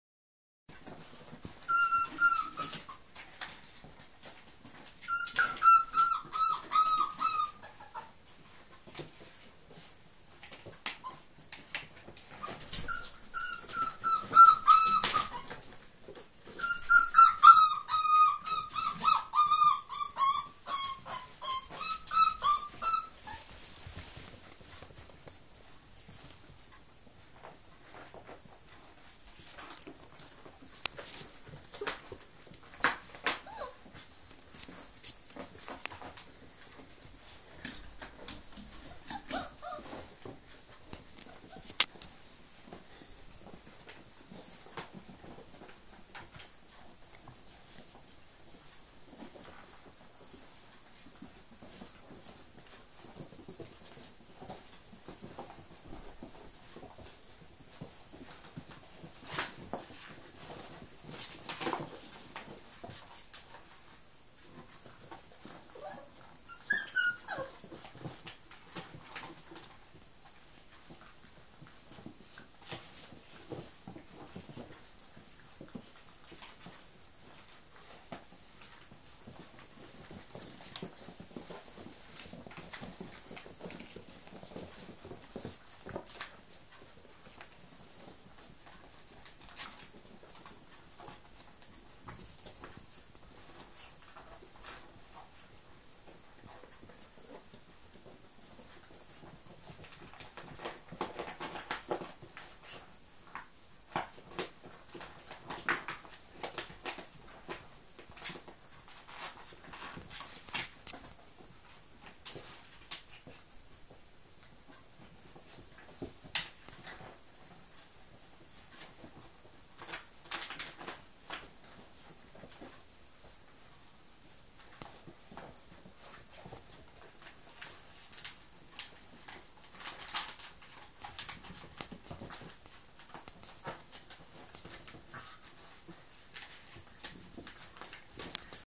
Naughty pups
Working Cocker spaniels For Sale. Only two left both chocolate brown male and female. Brought up in a family home with two noisy teenage kids, there mates and a cat. They will be glad to move away for a quiet life. Cracking pups mother and father both very quiet and loving.
The rabble went out and left the door open I wasn't quite quick enough with the camera as one of the pups had the cats collar on and I was "what in earth is going on". Notice the cat keeping up out of the way. She does go in amongst them and jumps out the way when she is fed up with being mauled and licked.